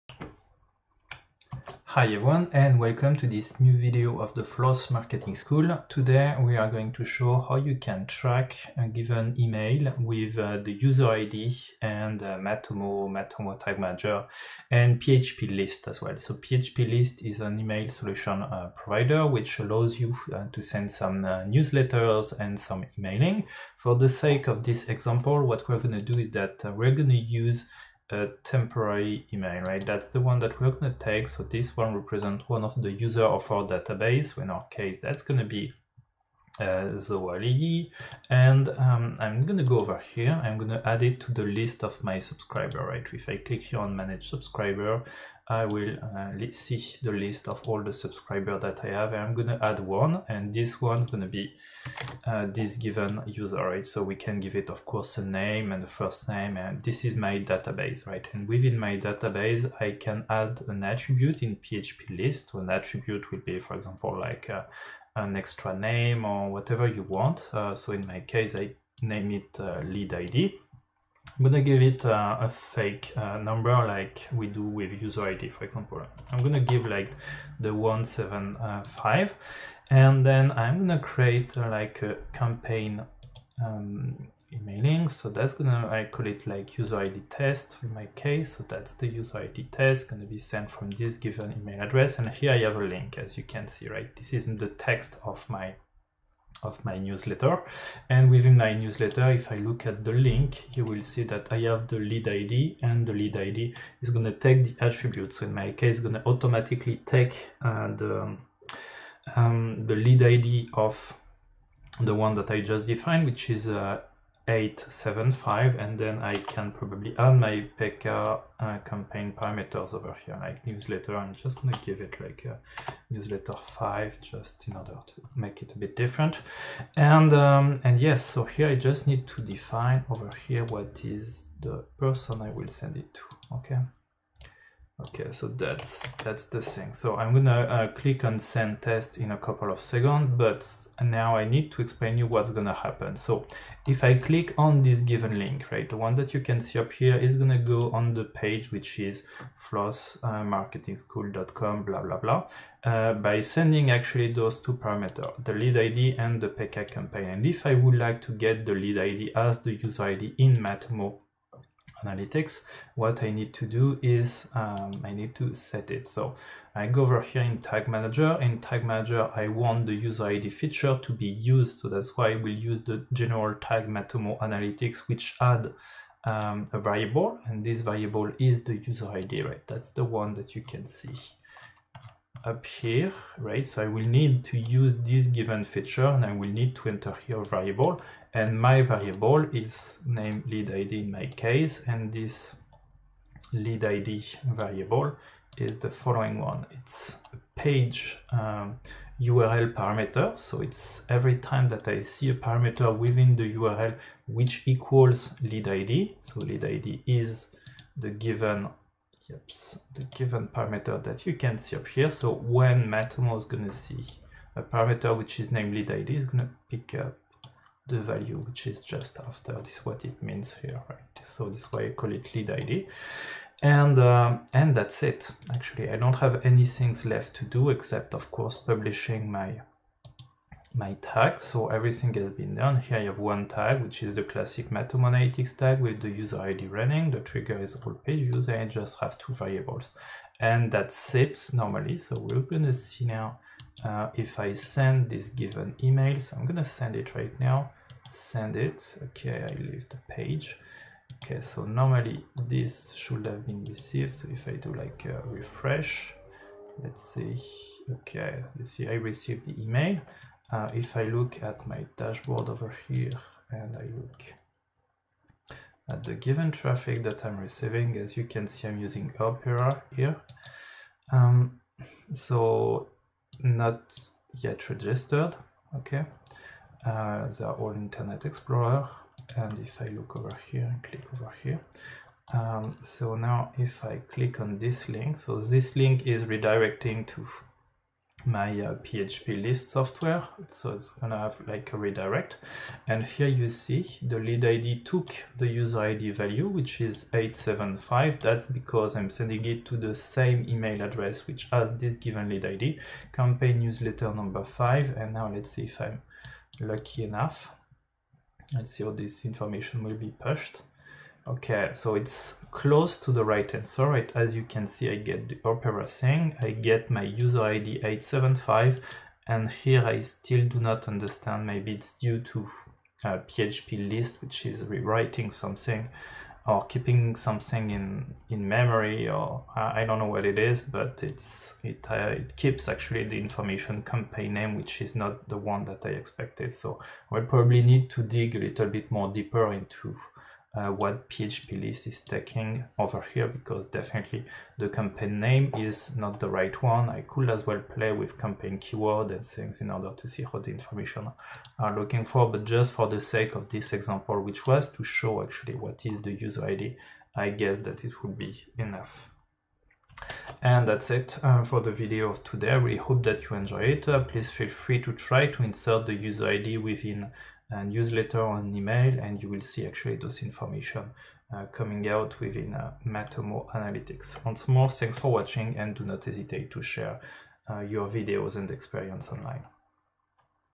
How to insert the user id within emails and track them with Matomo?
In this video, we are explaining how you can pass the user id through emails in order to track them within Matomo once they reach your website.